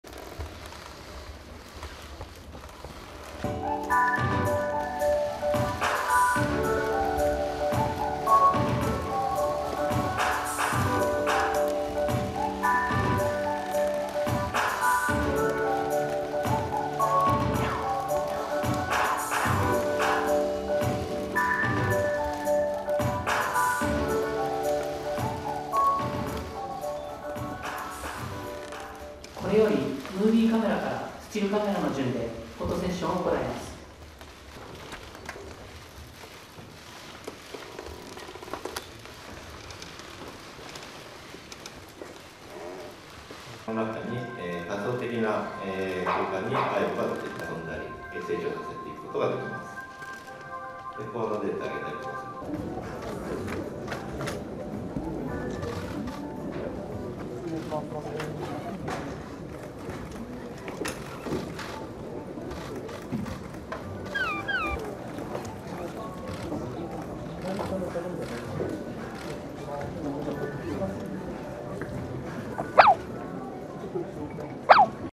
Sony kompaniyasi yangi robot kuchukni namoyish etdi / Japan's Sony announces new robot dog
Japan's Sony Corp unveiled on November 1 a new robot dog called "Aibo", a successor to its former popular robot dog with the same name that went defunct in 2005. The robotic man's best friend is 180mm wide, 293mm tall, equipped with OLED in its eyes, and runs for about two hours after a full charge of three hours. It has two cameras that use facial recognition to identify its owner, and 22 actuator motors for movement. Owners can also sync "Aibo" with a smartphone application called "My Aibo", which allows them to play with the robot dog remotely. Preorders begin later today for $1,738 plus a basic monthly network plan. Shipments will start on January 11, 2018. Sony's announcement came a day after it forecast record earnings, leading to a nine-year high in its share price.